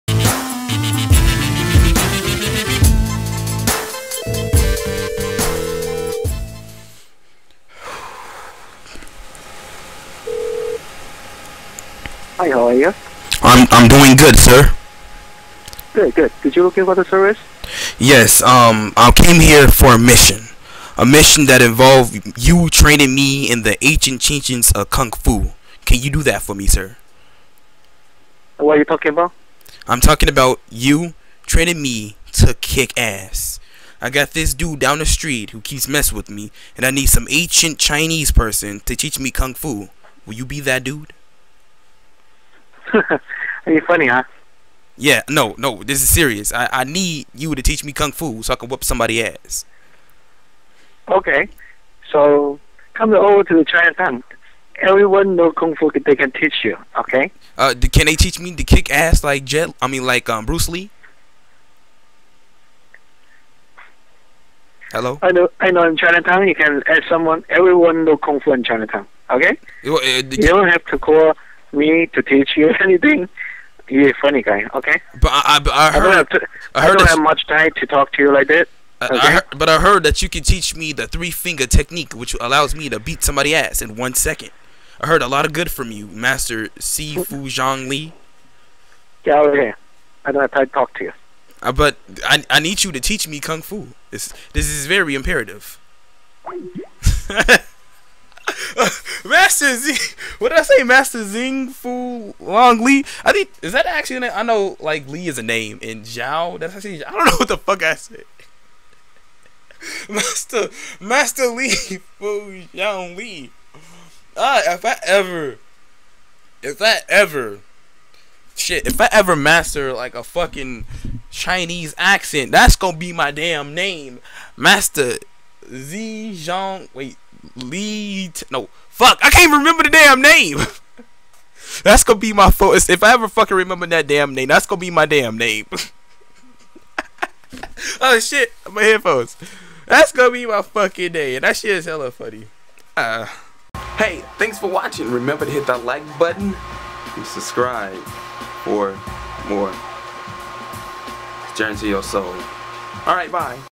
kung fu lesson prankcall
so basically i was on backpage and i said hmm imma call an asian prostitute agency looking for kung fu lessons so i did it